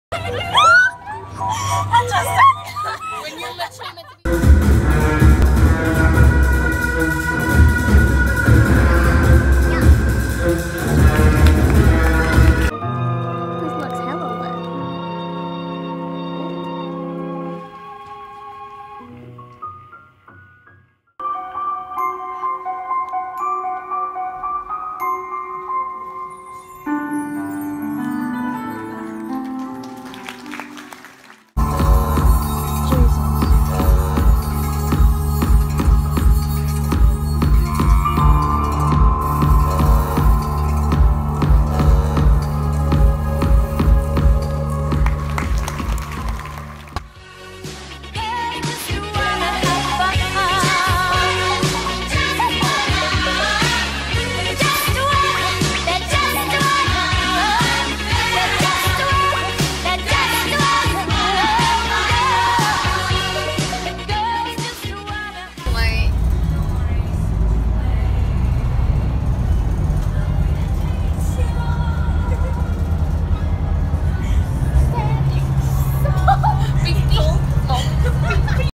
GIRLS NIGHT + CIRKOPOLIS 2017
Girls night out was hella eventful and adventurous! Short Vlog just cause my phone was on low battery the whole night 👀😩